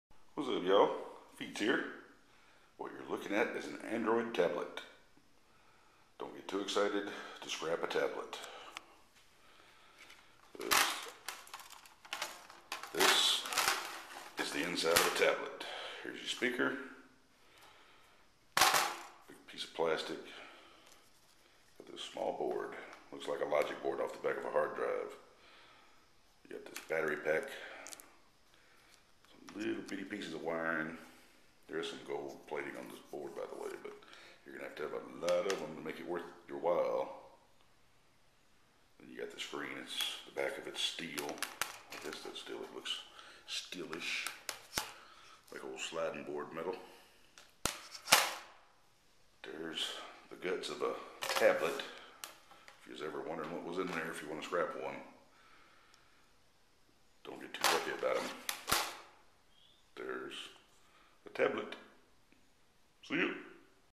Scrapping An Android Tablet: Gold Recovery
Don't get too excited because you will need a lot of them to make any money from recovering the gold.